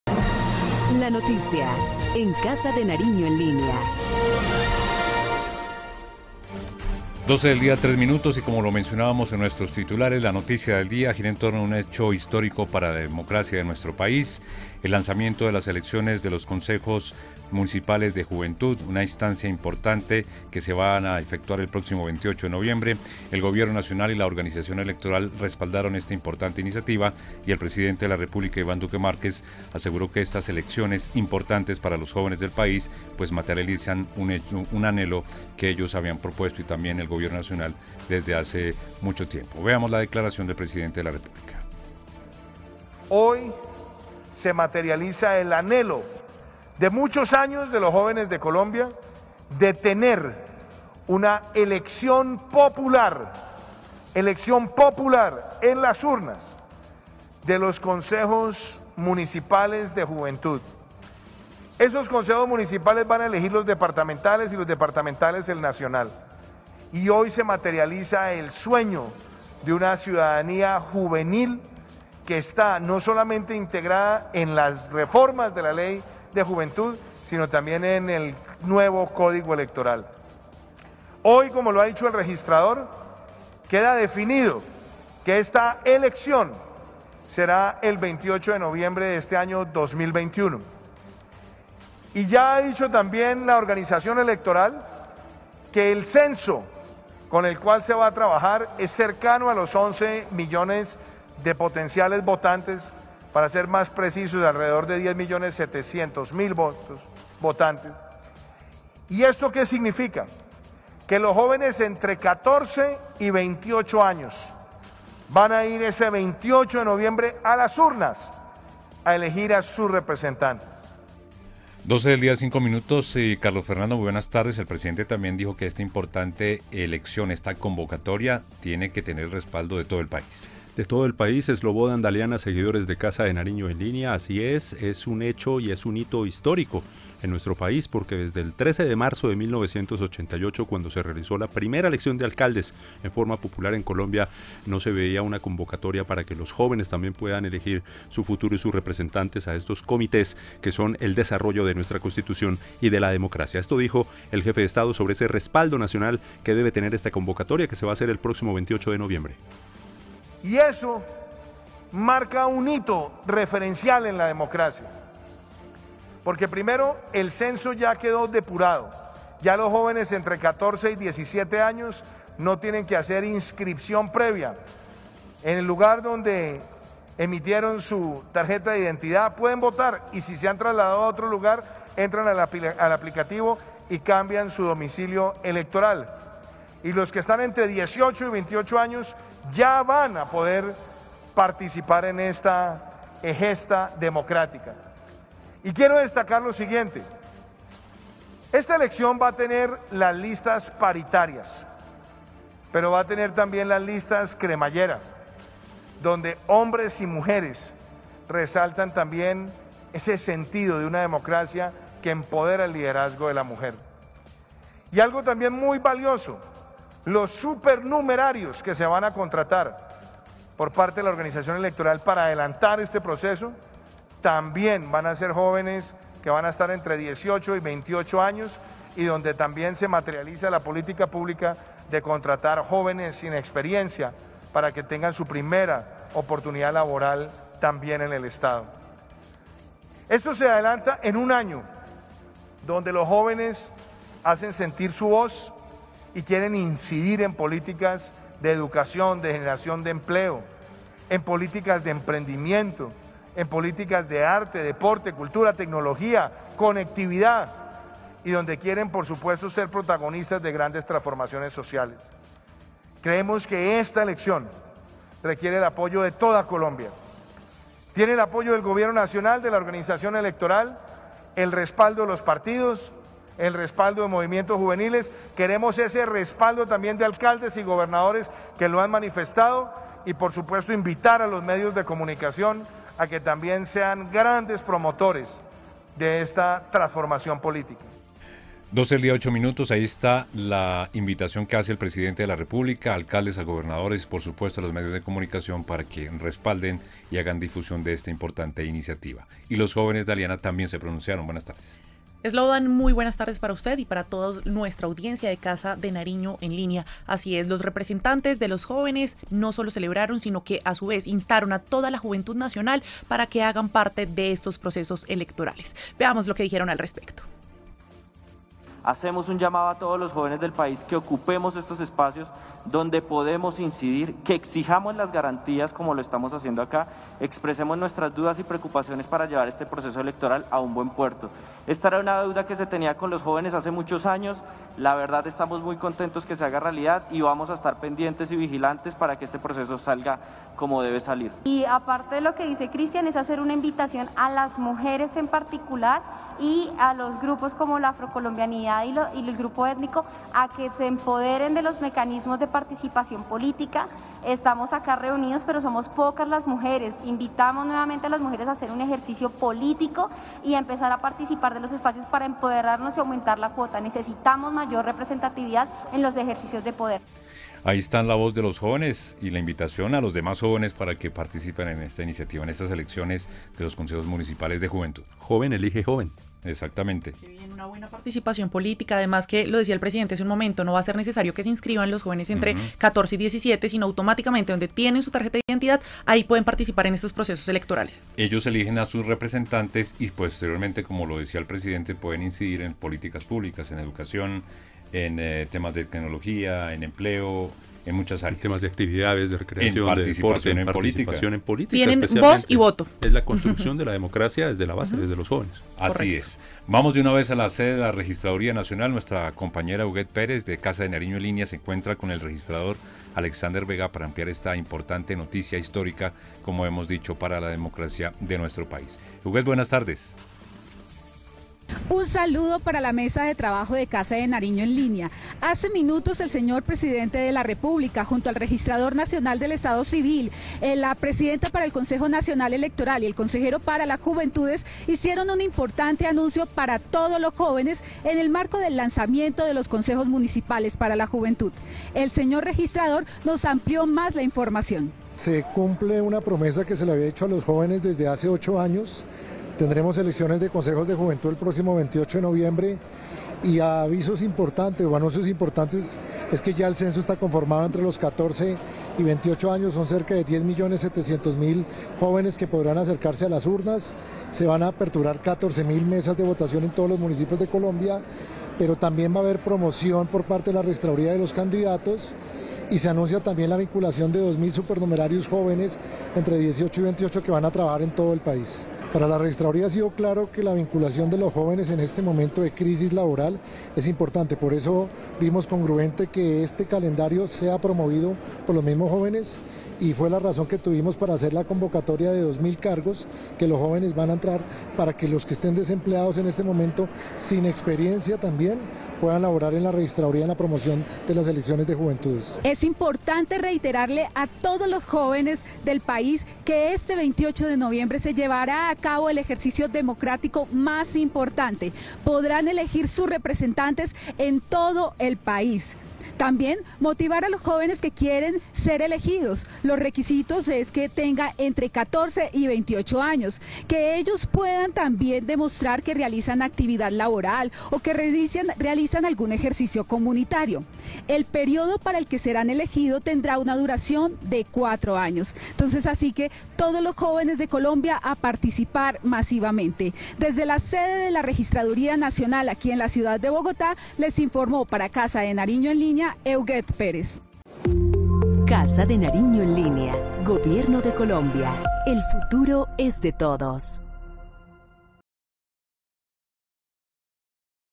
Noticia del Día: primera elección de consejos de juventudes fue presentada por presidente Duque
Noticia del Día: primera elección de consejos de juventudes fue presentada por presidente Duque - Casa de Nariño en Línea